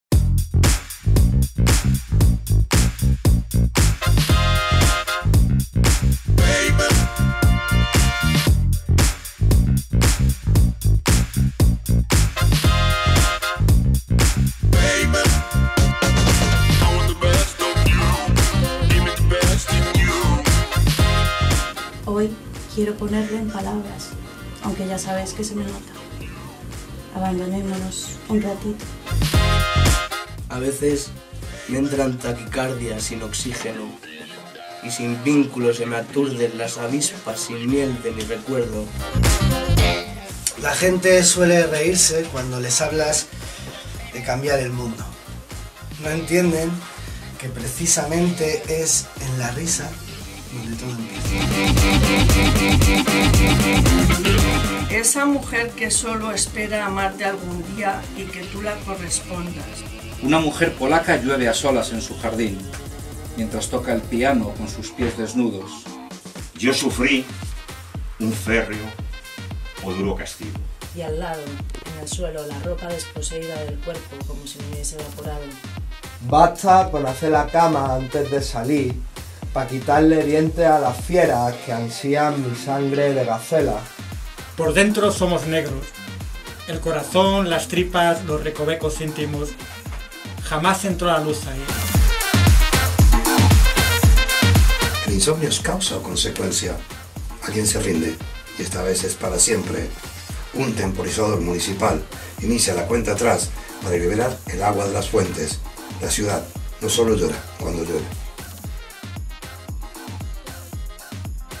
TROTAVERSO en MAYO
Seguimos con las actividades poéticas del año 2023 con un  Trota VERSO, en mayúsculas, dentro de las actividades de CRUZA CARABANCHEL
La librería El Trotajueves inicia su participación en este gran evento cultural con una de sus actividades más importantes dentro de su programación habitual.
Como es habitual  nuestro compañero y amigo CARLOS SALEM volvió a llenar la librería de lírica y rima en este barrio que comienza a tener su lugar entre la cultura europea.